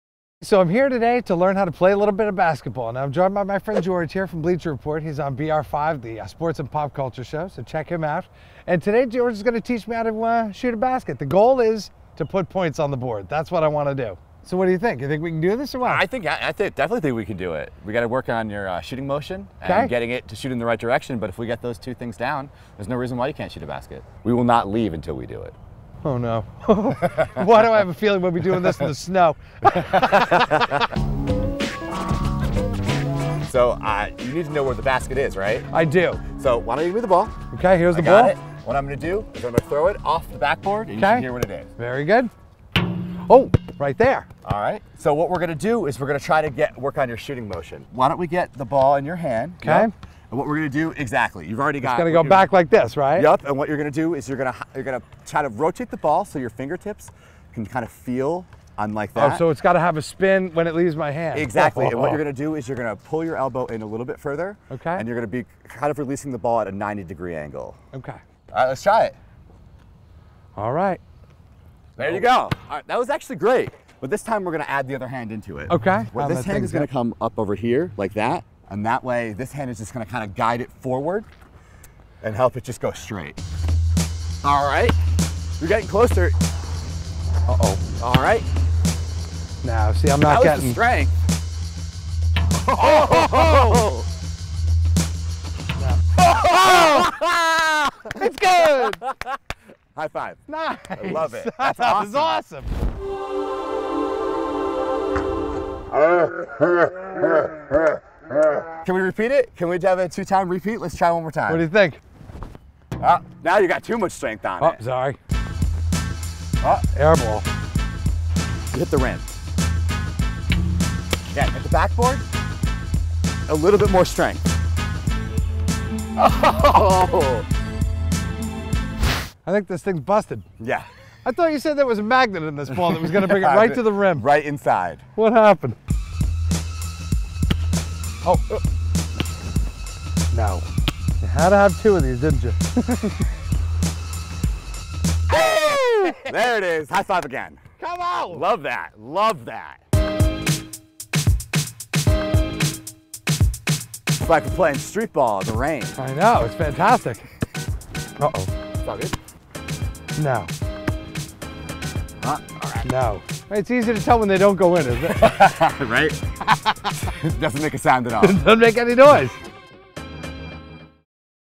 Blind Man Learns How To Shoot A Basketball
Tommy, who has been blind since birth, learns how to play basketball. George Wasgatt, from BleacherReport You Tube channel, attempts to show Tommy how to score a basket.

CREDITS:

THE TOMMY EDISON EXPERIENCE
Starring Tommy Edison